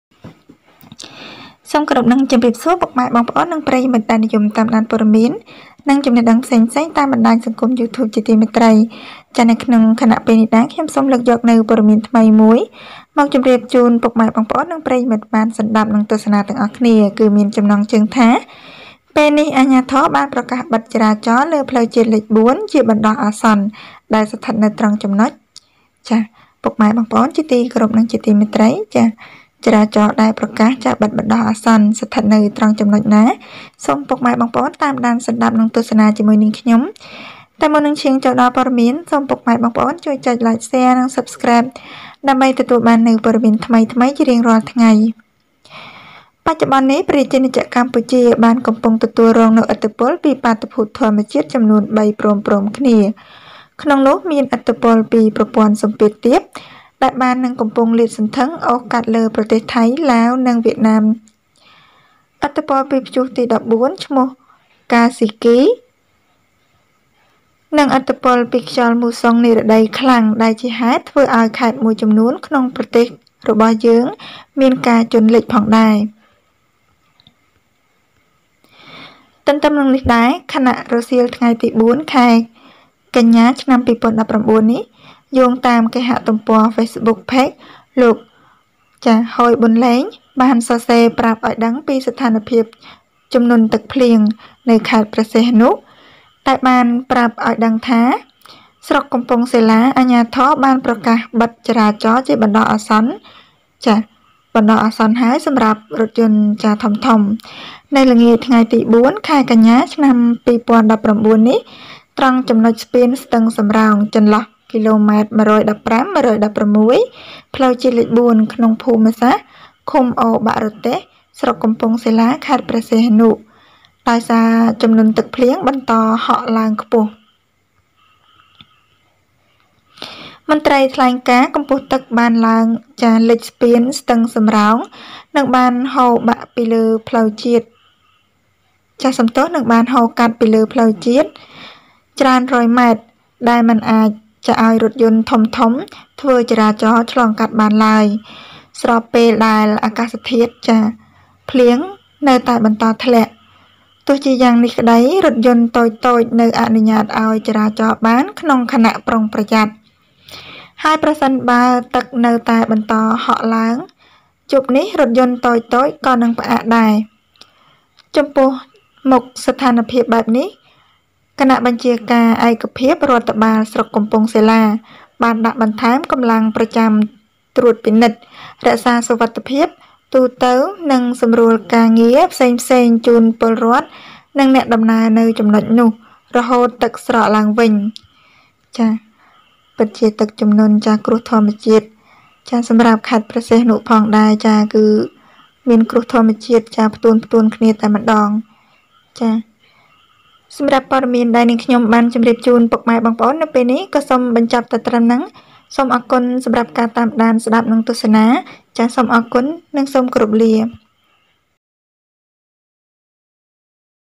ពេលនេះអាជ្ញាធរបានប្រកាសបិទចរាចរណ៍លើផ្លូវជាតិលេខ៤ ជាបណ្តោះអាសន្ន ដែលស្ថិតនៅត្រង់ចំណុច
Authorities have now announced a temporary blockade of National Road 4 at the